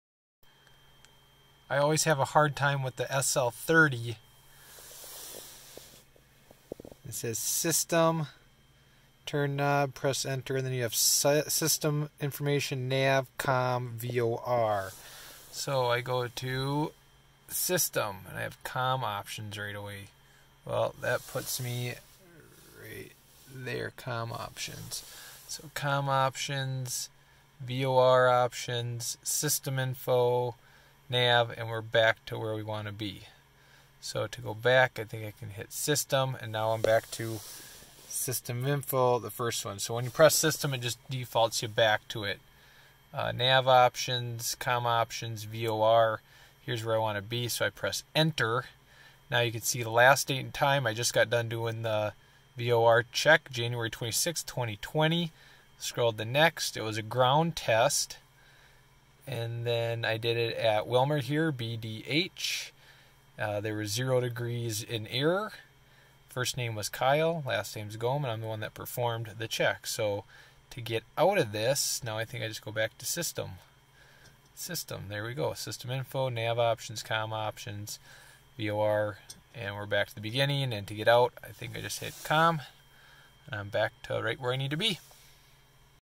Garmin SL30 VOR VOT Check
Always do your VOR check every 30 days. You never know when the government is going to jam the GPS.